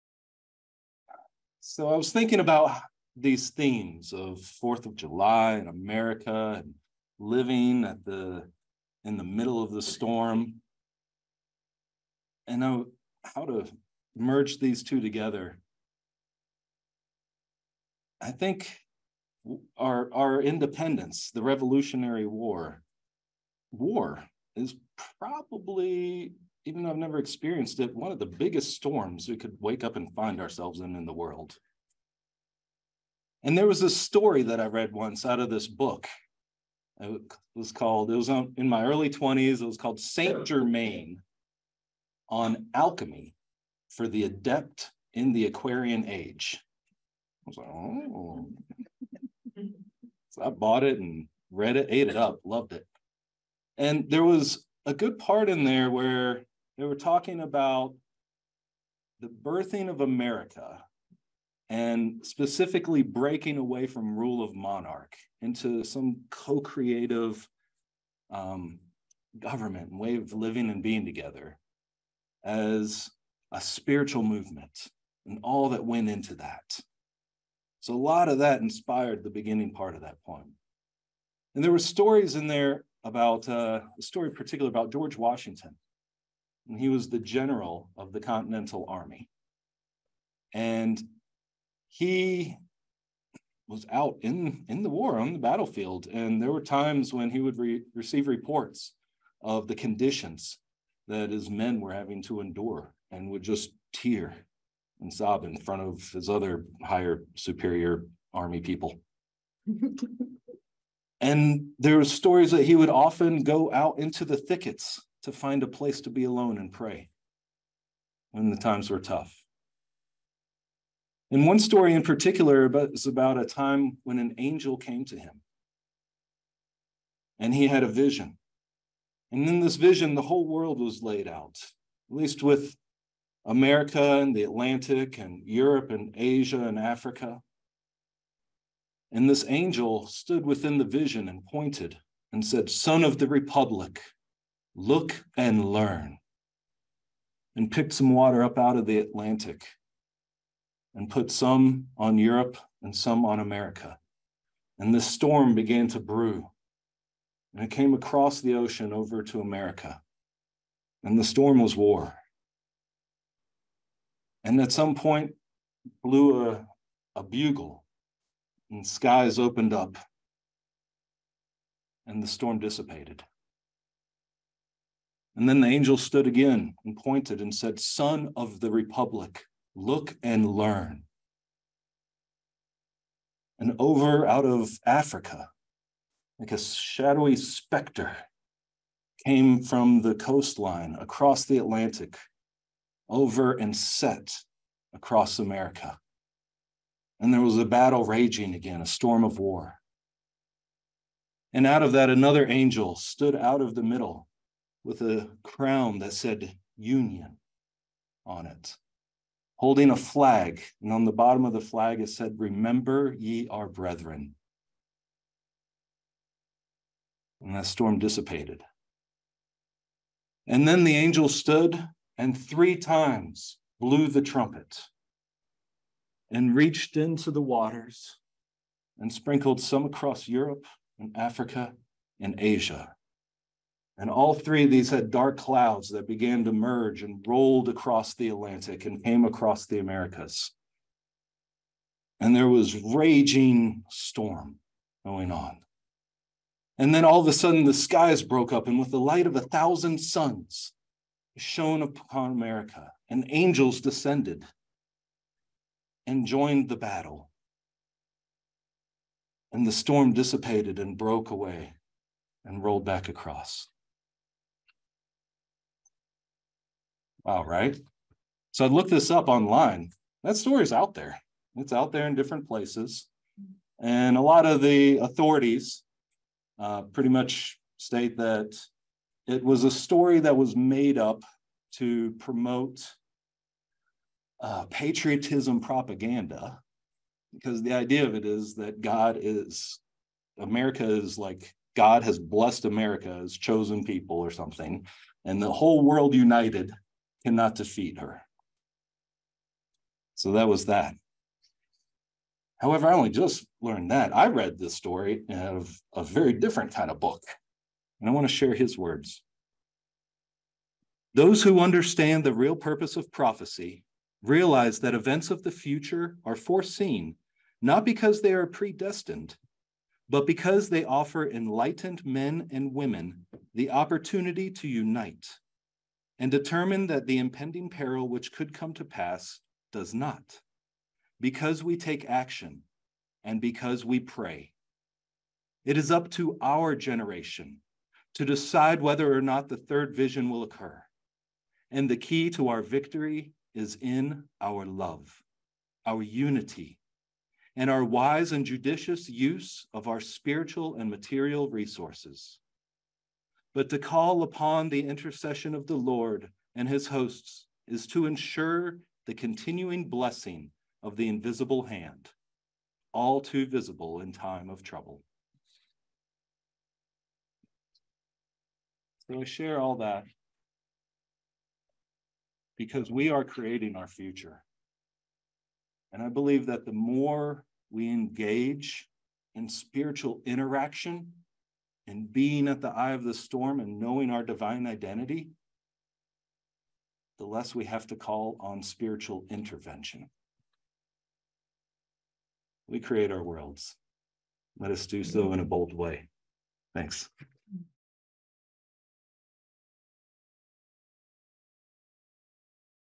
Gary Goodhue: America's Divine Identity
What is the spiritual foundation of The United States? Gary reflects on the spiritual vision founding father George Washington had for this land.

Attunement Practitioner Gary Goodhue: "America's Divine Identity" excerpt from our Sunday service on July 2nd,  2023  at Sunrise Ranch in Loveland, Colorado.